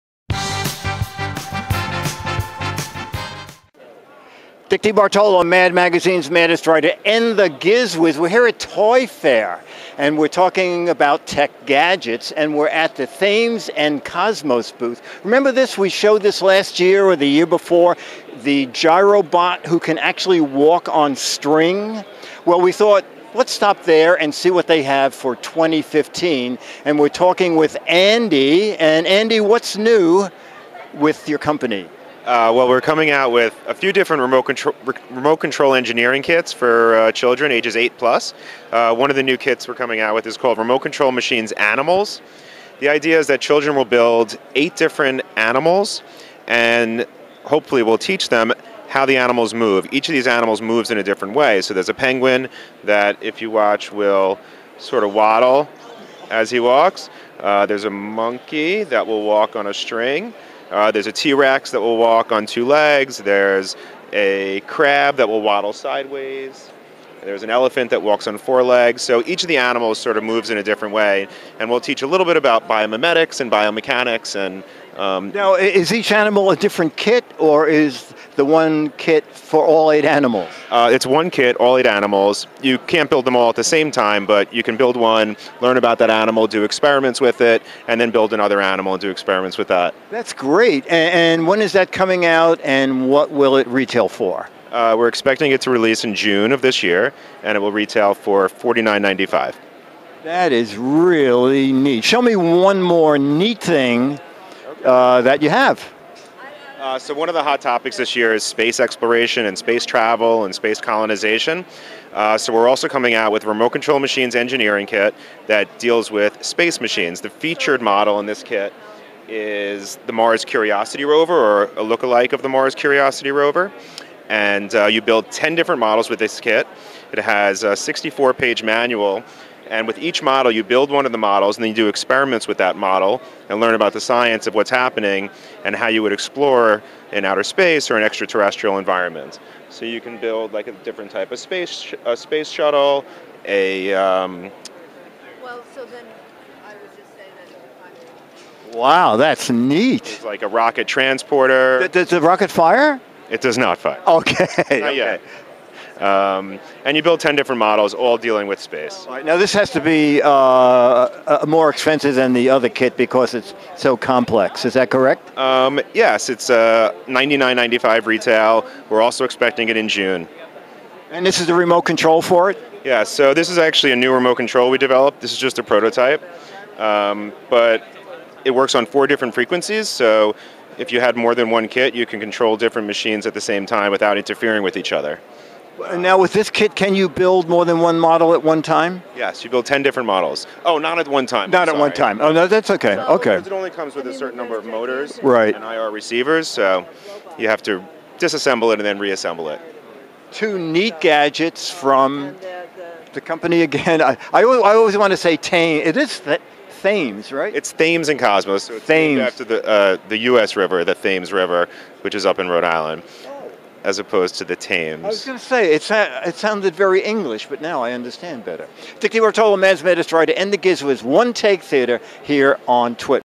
From Toy Fair 2015 two fun tech building kits for kids 8+.
These two (educational) remote control kits are from Thames & Kosmos. Don't let kids hear the 'educational' part, but they can see that they are really fun. Both kits should be on retail shelves in June, 2015. Once they're widely available my guess is you'll be able to search the web for discounts. I know the kit we featured in the past, the Gyrobot kit, is available at a discount  online.